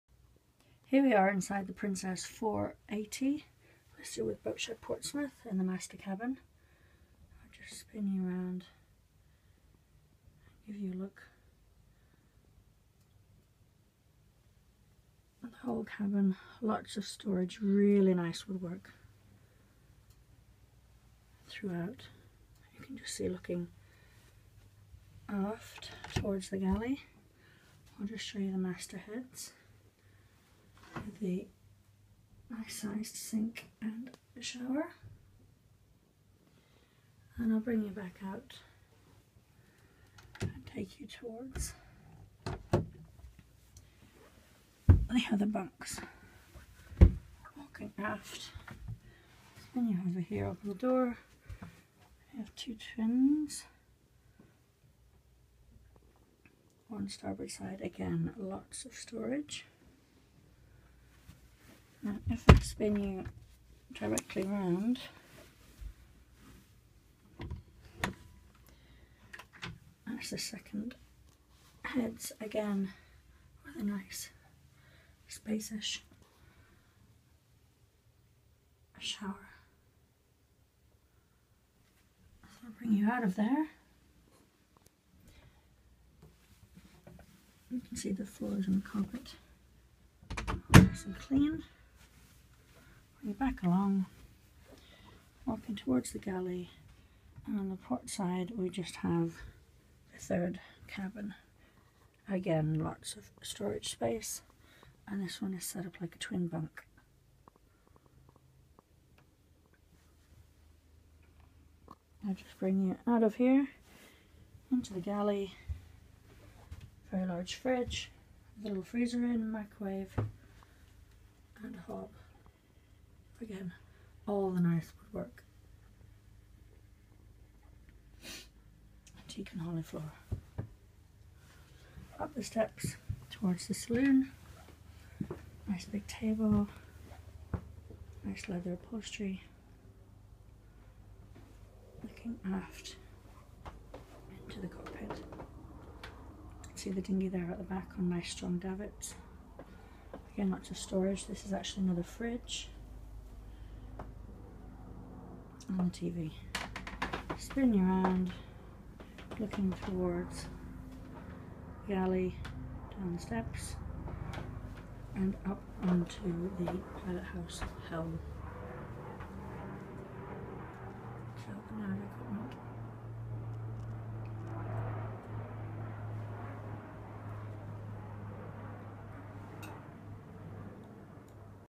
Princess 480 - Boatshed.com - Boat Ref#167246
Princess 480 for sale with Boatshed Portsmouth Photos and video taken by Boatshed Portsmouth